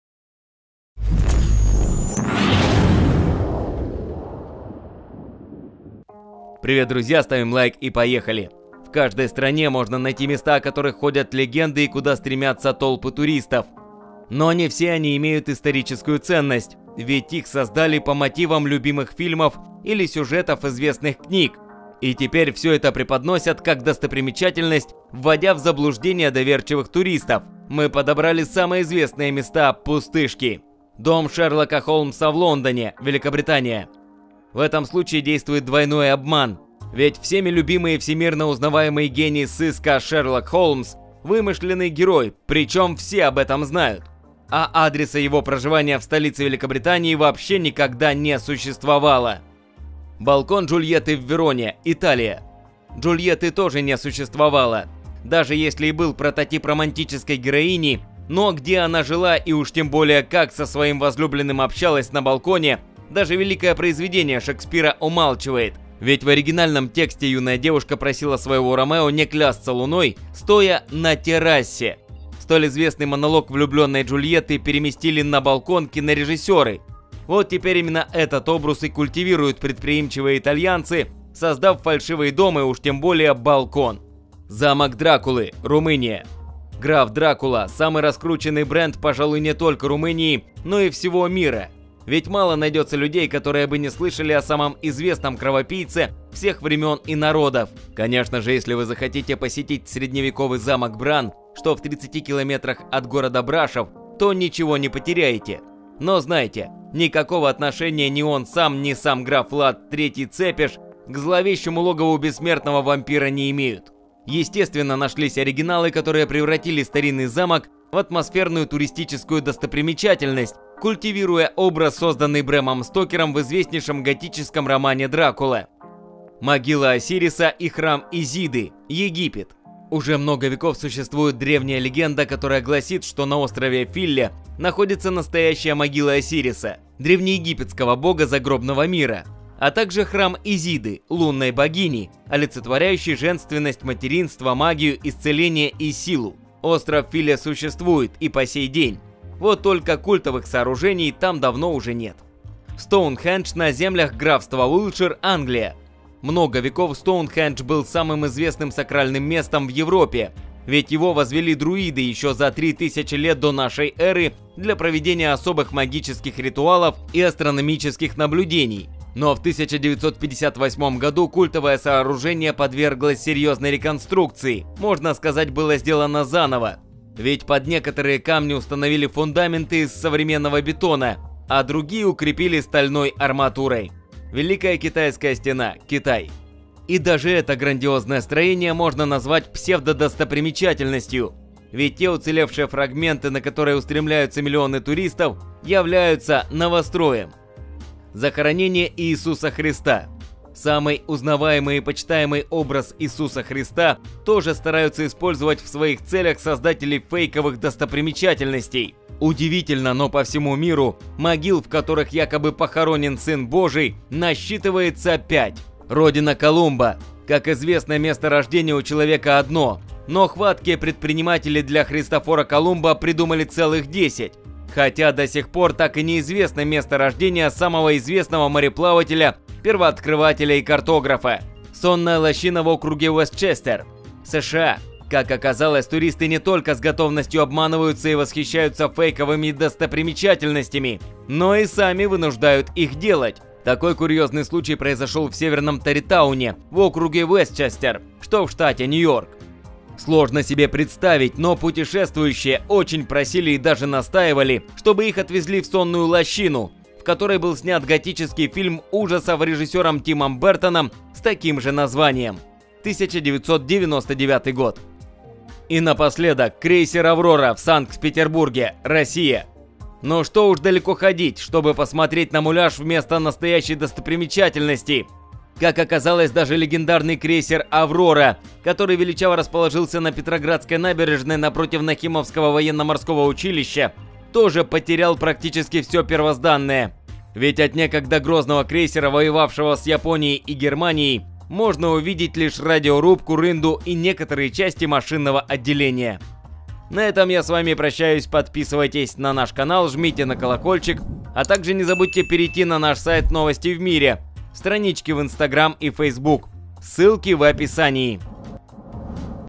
10 выдуманных достопримечательностей, к которым, невзирая ни на что, стремятся толпы туристов
В каждой стране можно найти места, о которых ходят легенды и куда стремятся толпы туристов. Но не все они имеют историческую ценность. Ведь их создали по мотивам любимых фильмов или сюжетов известных книг, и теперь все это преподносят, как достопримечательность, вводя в заблуждение доверчивых туристов. Мы подобрали самые известные места-пустышки. Подробности в нашем ролике!